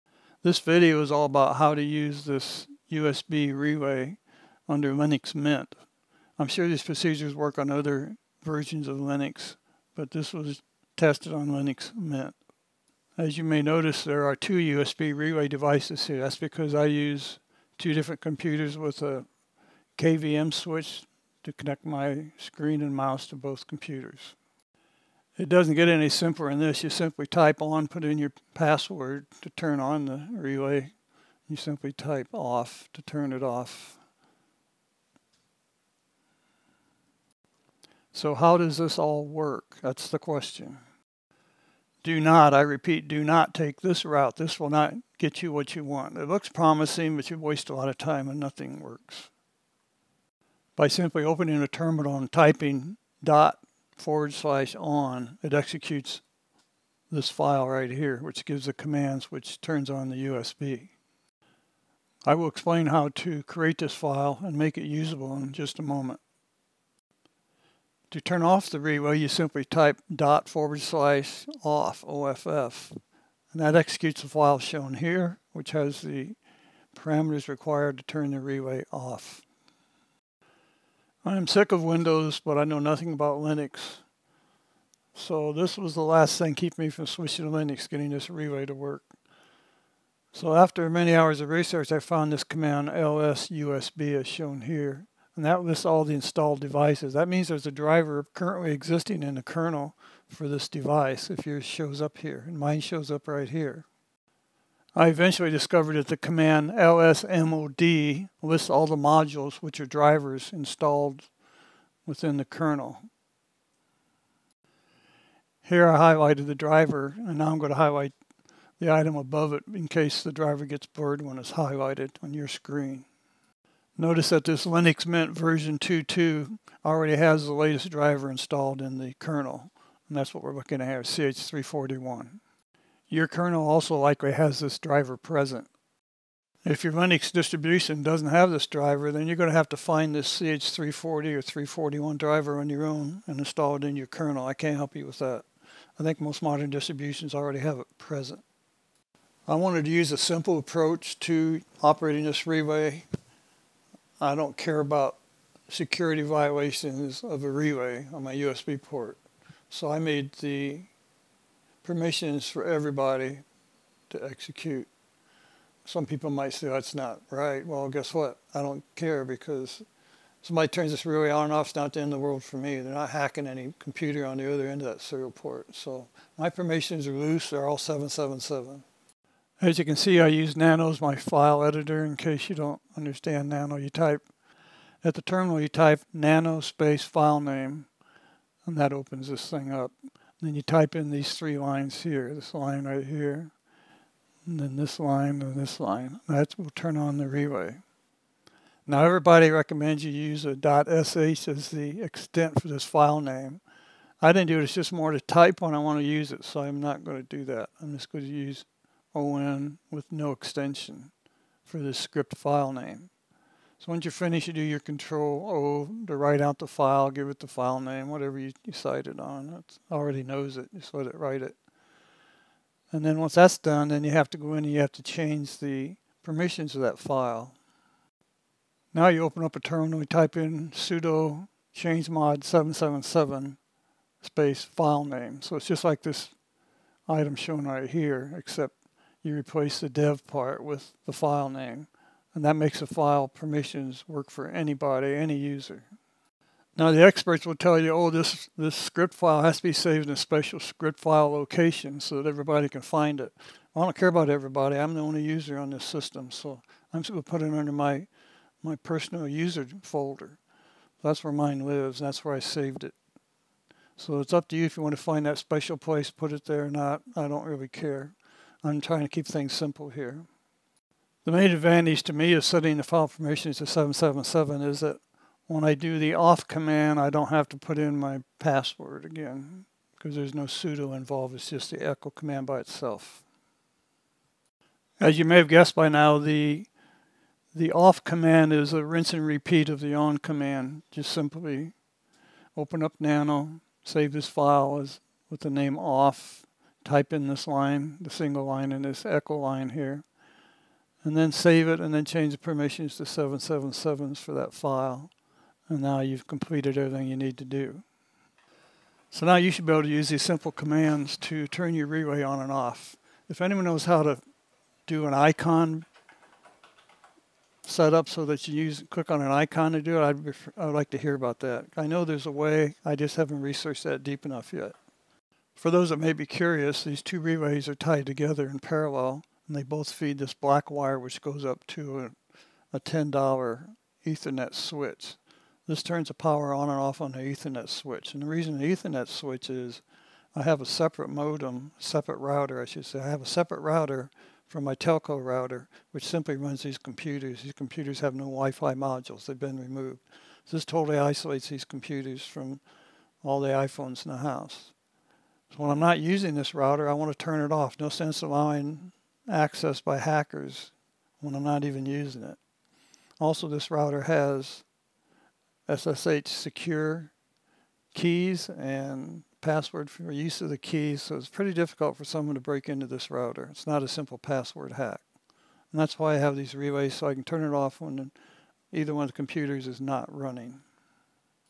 Linux control of USB relay simplified
Explains in great detail a simple approach to turning usb relays on or off.